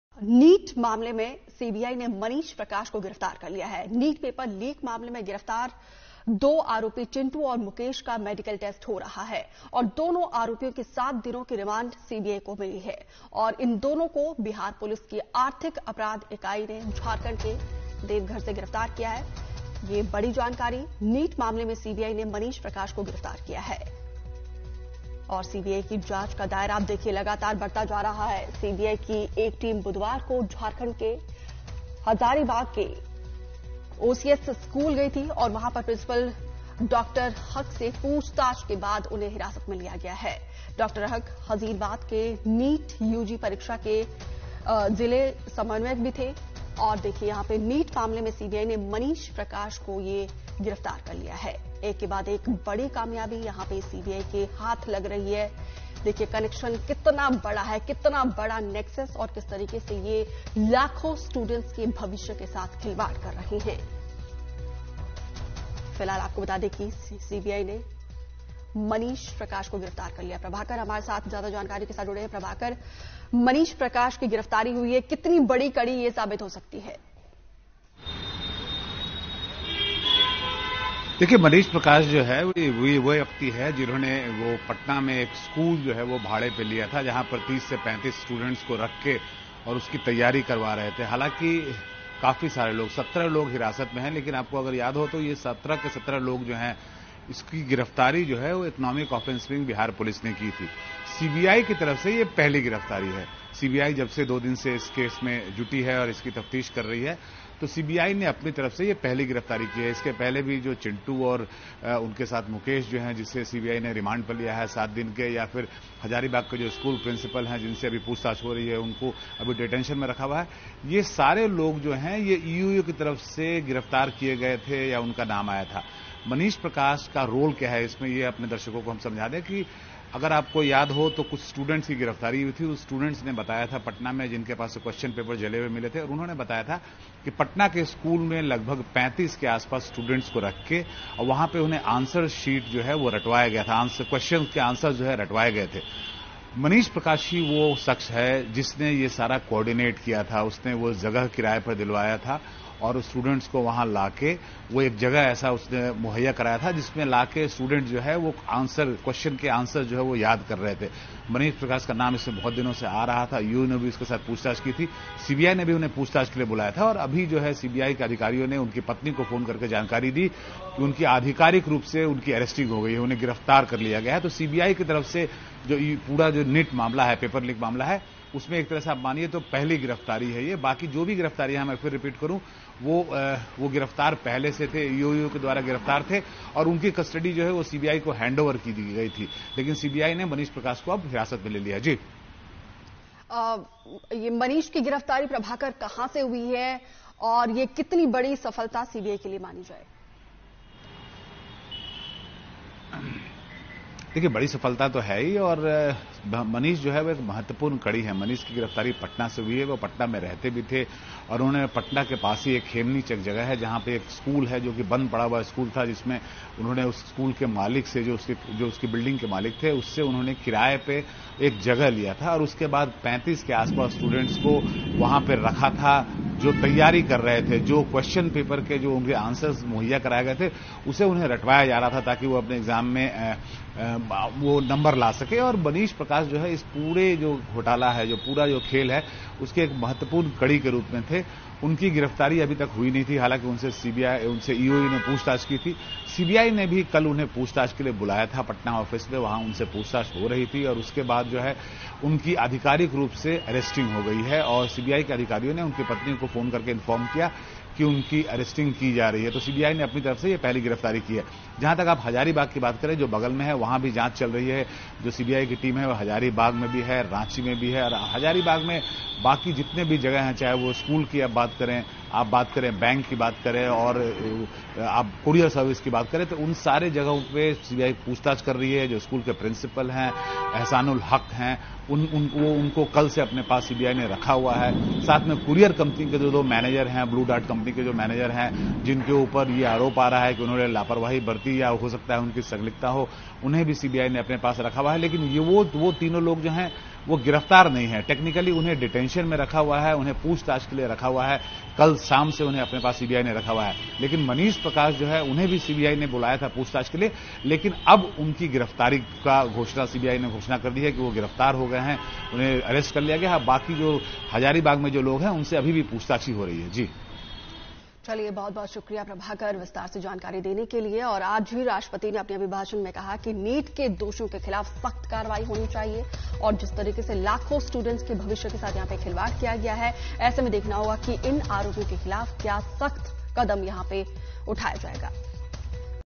NEET पेपर लीक में CBI ने की पहली गिरफ्तारी
आखिर कौन है मनीष प्रकाश क्या कनेक्शन है पेपर लीक मामले मे .